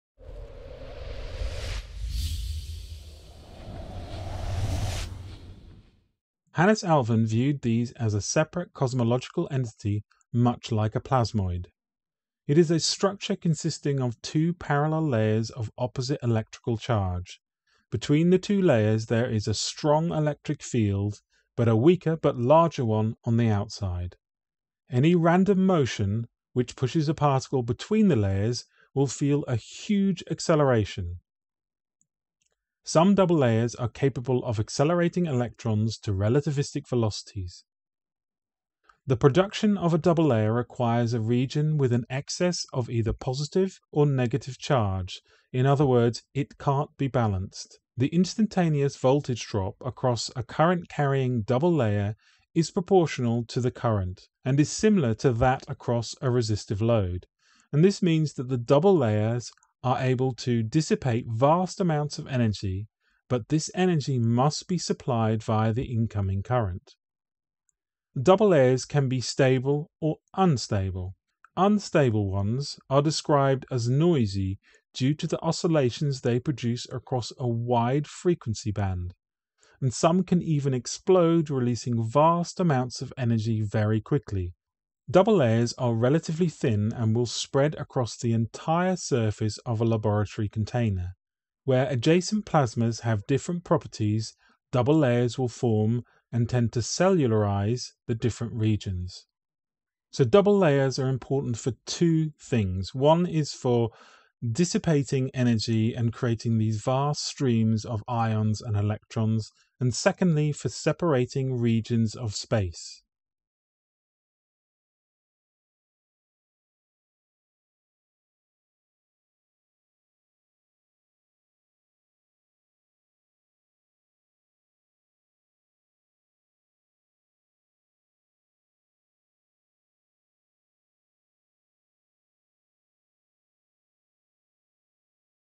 Understanding Plasma Double Layers in 120 Seconds
The Electric Universe and Plasma Universe use many terms which at first may seem confusing.  In this video we will explain what a plasma double layer is.  For a more detailed version please see the suggested video at the end.